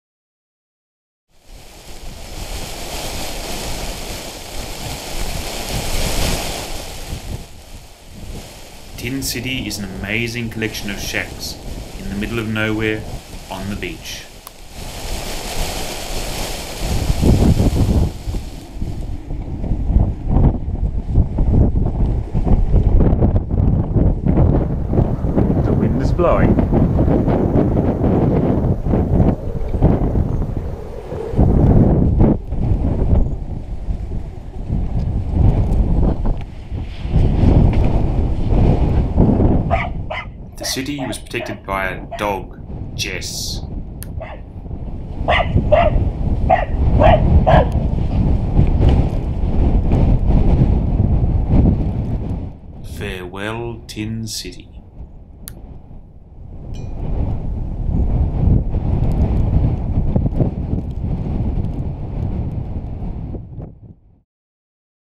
Tin City
A visit to Tin City - a surreal collection of tin sheds in the middle of the sand dunes in Stockton, NSW, Australia.